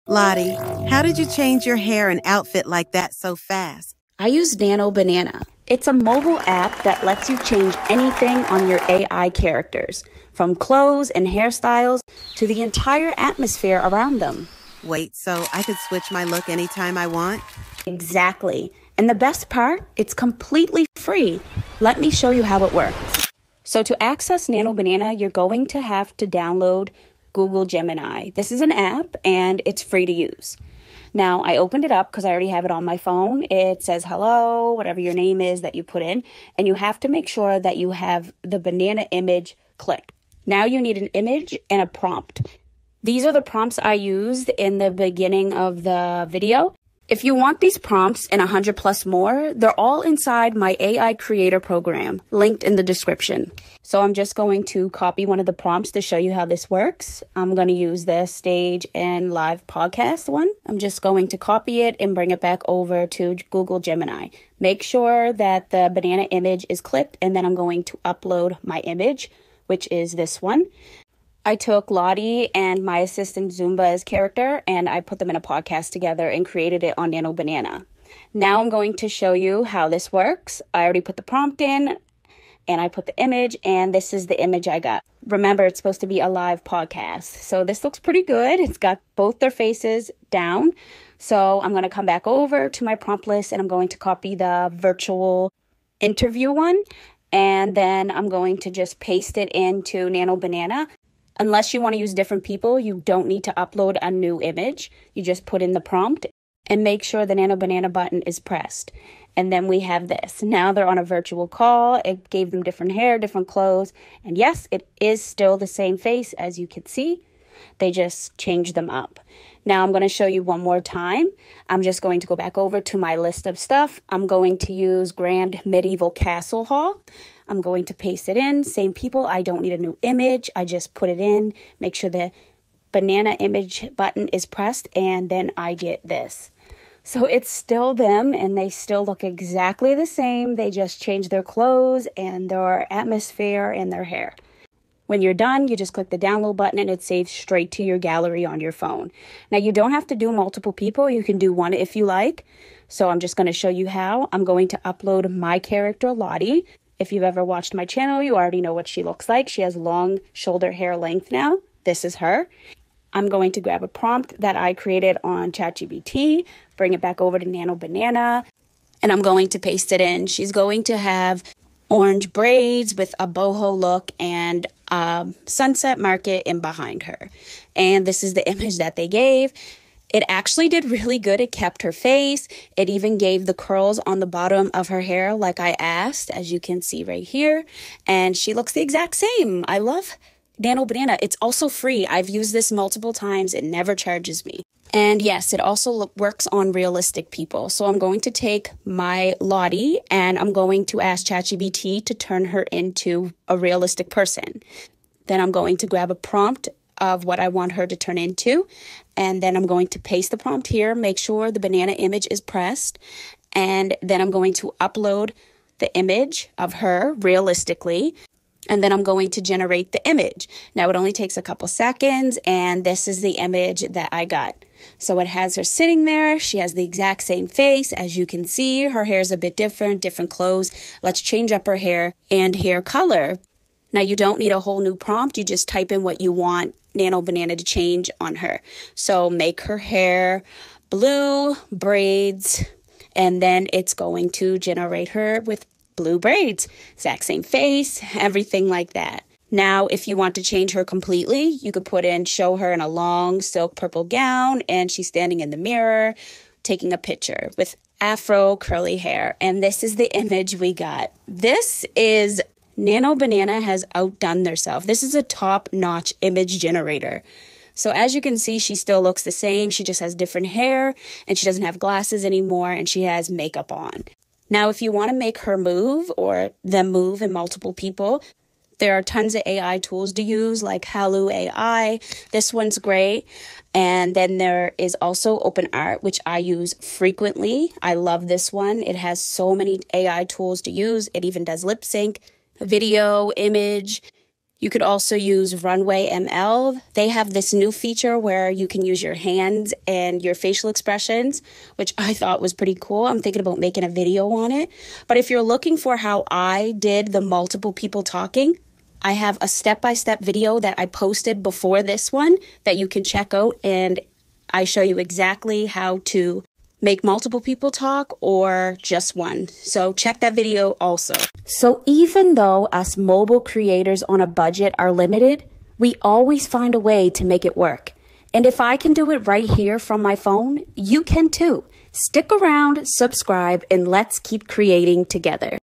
How To Use Nano Banana FREE On Mobile | Consistent Characters
Nano Banana is now completely free to use on mobile, and in this video I’ll show you exactly how to get started. You don’t need a computer or expensive software — just your phone. Discover how to unlock Nano Banana for free, explore its features, and see why so many creators are excited about this update. If you’ve been waiting to try Nano Banana without paying, this is your chance to see how it works step by step, all from your mobile device.